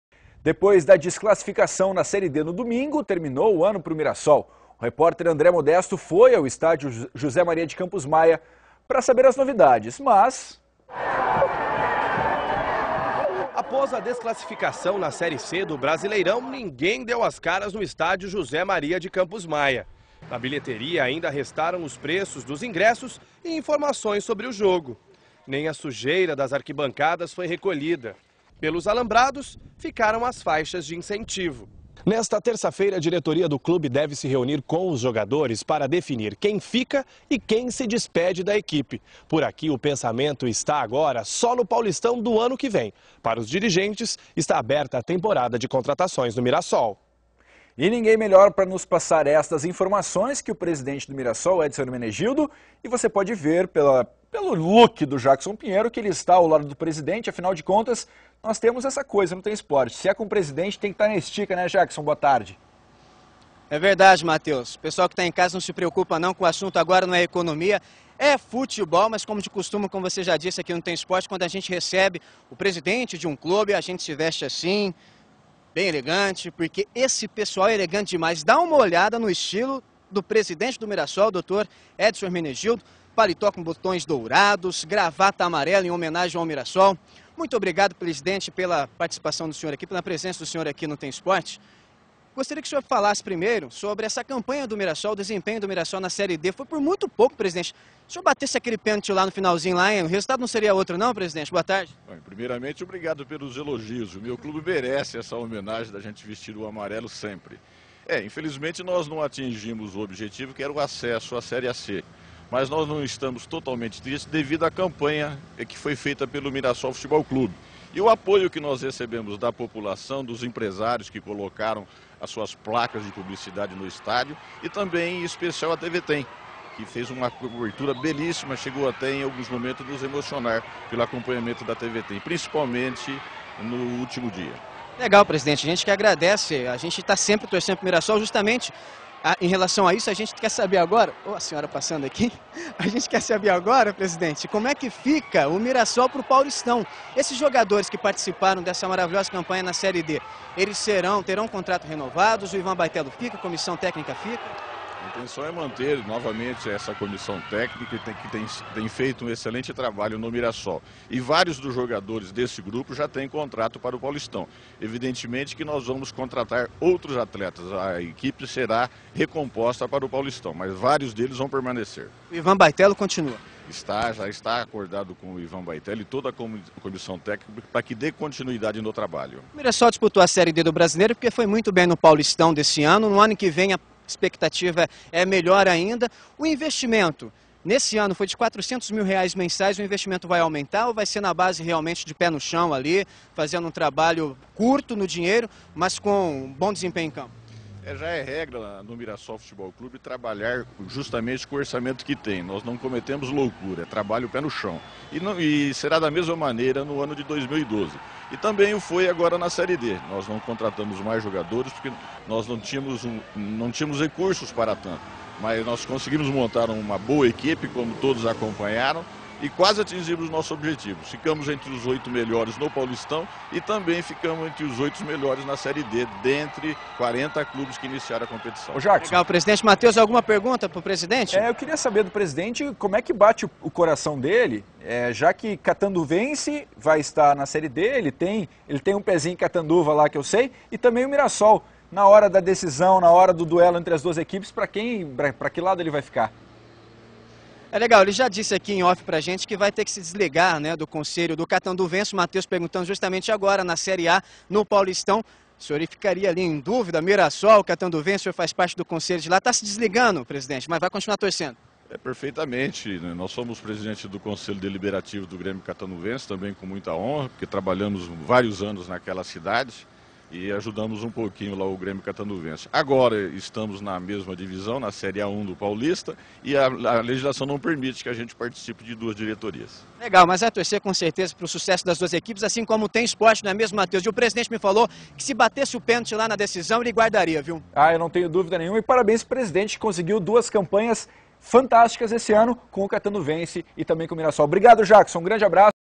Entrevista Presidente Mirassol FC Edson Ermenegildo
Entrevista com o Presidente do Mirassol Futebol Clube, Edson Ermenegildo sobre a campanha na Série D do Brasileiro.